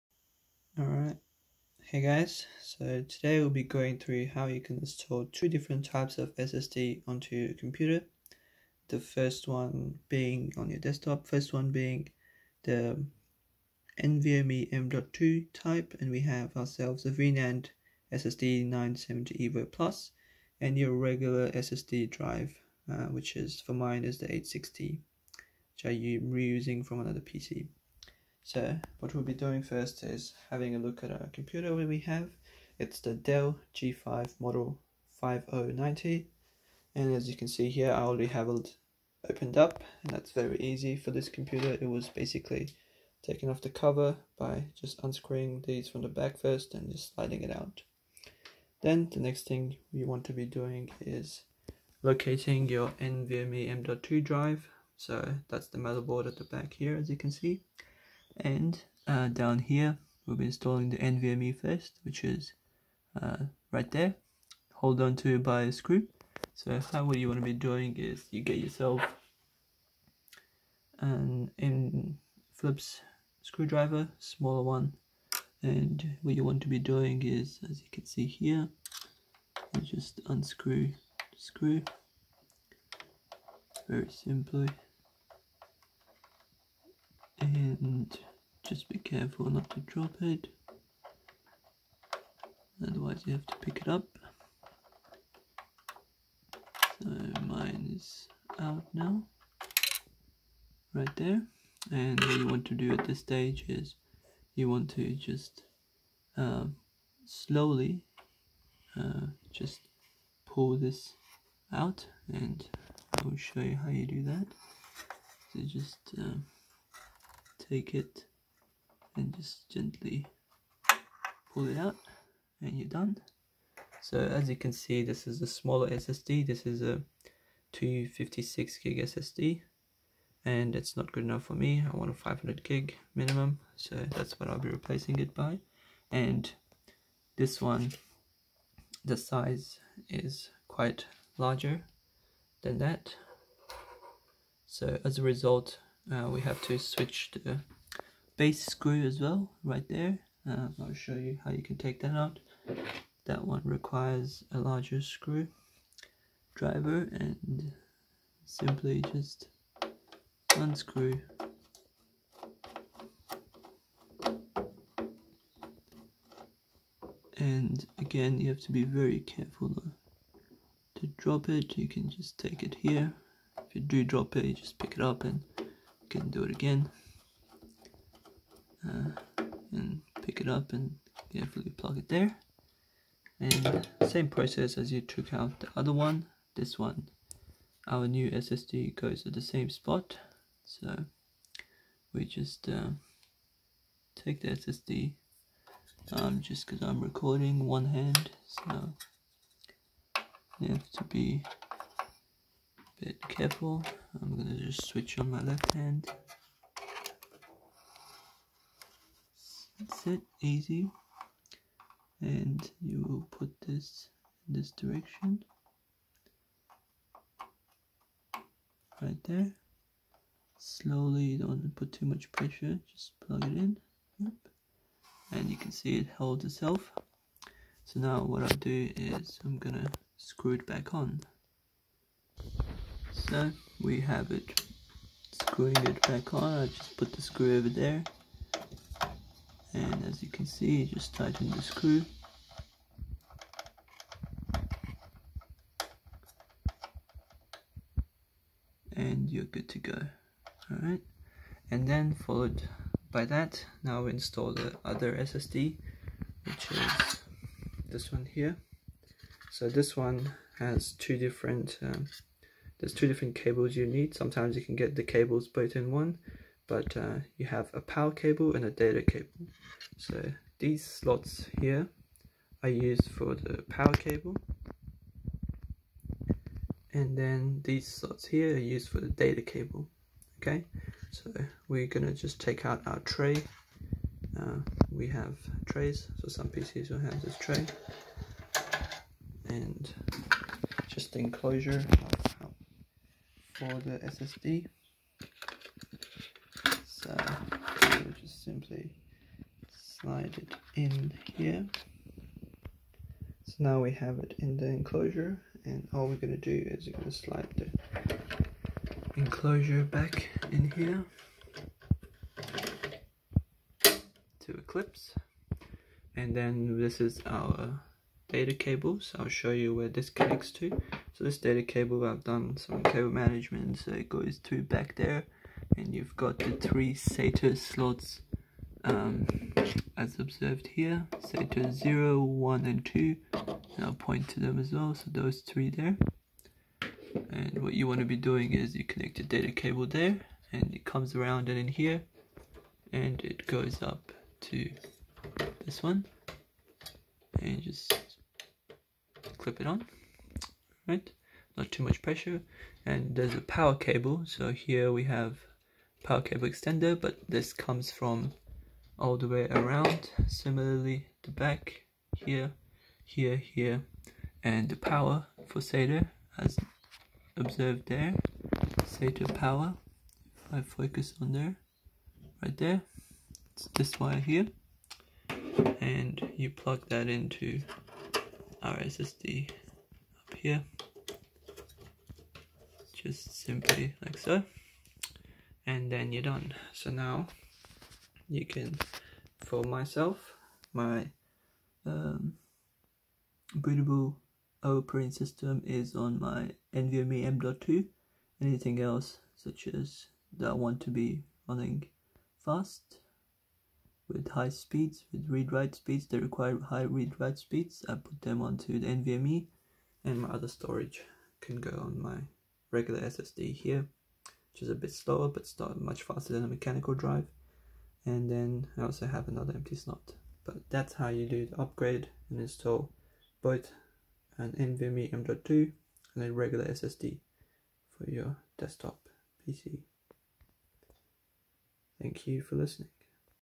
Installing NVMe M.2 SSD and Regular SSD Hard Drive
Installing the 2 different types of SSD drives on the DELL G5 5090.

1. Samsung V-NAND SSD 970 EVO Plus NVMe M.2 500GB
2. Samsung SSD 860 EVO 500GB

The upgrades will provide much better read-write speeds compared to the stock SSD and mechanical hard drives.